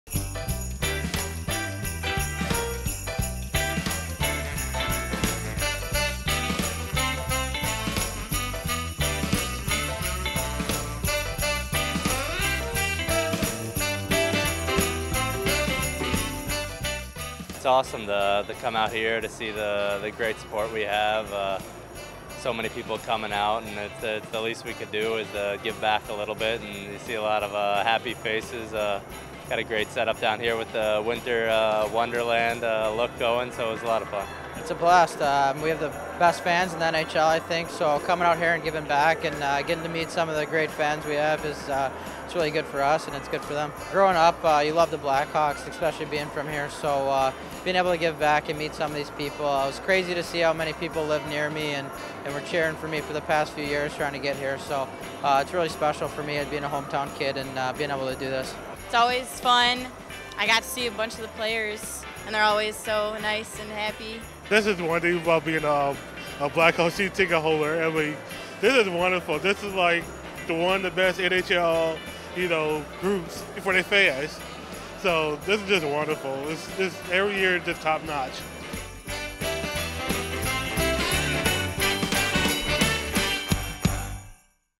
Blackhawks spend time with fans at Winter WonderFest
Blackhawks players singed autographs and chatted with season ticketholders at Navy Pier's Winter WonderFest.